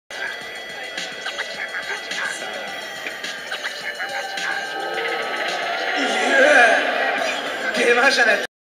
Yuki Kadono almost lands a triple cork in practise.
Yuki Kadono almost gets a triple cork to stick in practise for the Toyota Big Air 2013 in Sapporo Dome.